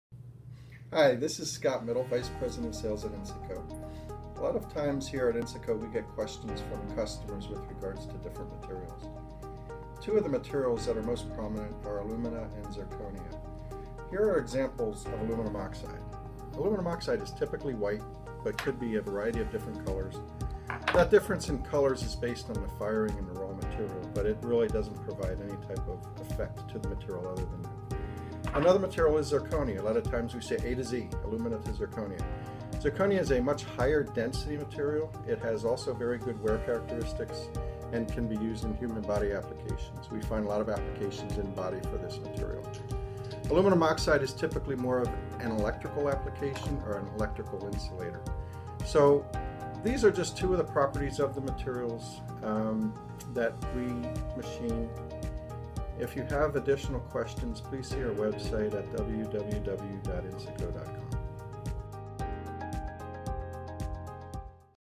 Zirconia Alumina vs Alumina Oxide - Breakdown & Comparison - With music
This video explains zirconia alumina vs aluminum oxide.

Insaco Inc.